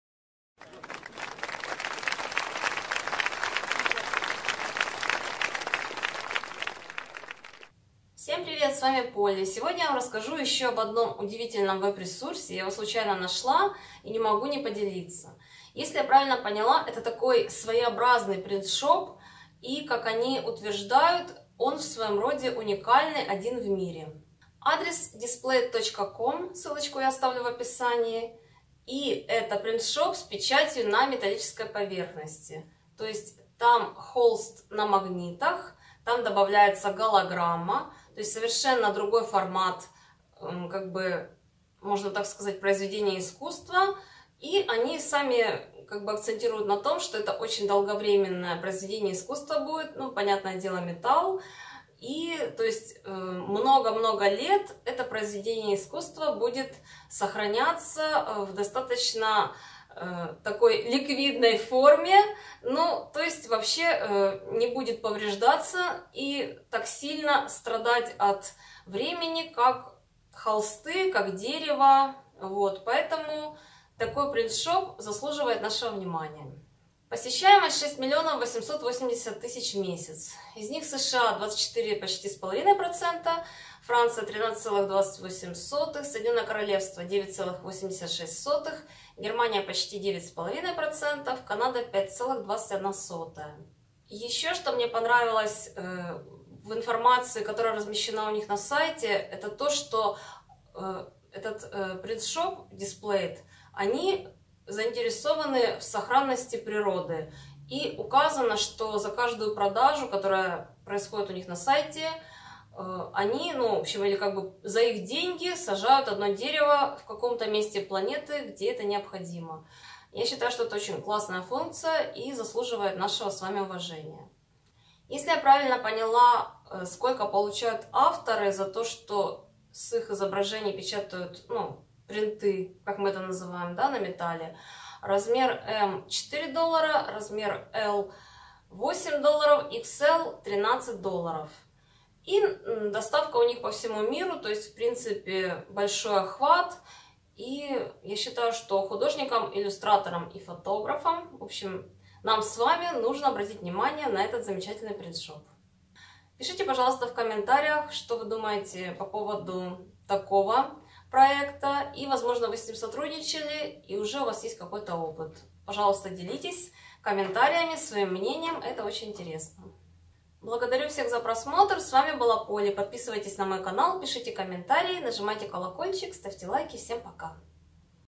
Принтшоп Displate. Заработок в интернете для художников и иллюстраторов
Принтшоп Displate. Заработок в интернете для художников и иллюстраторов. Обзор Poly

00:08 о Displate
01:16 посещаемость на Displate
01:34 о защите экологии со стороны Displate
02:05 о заработке авторов на Displate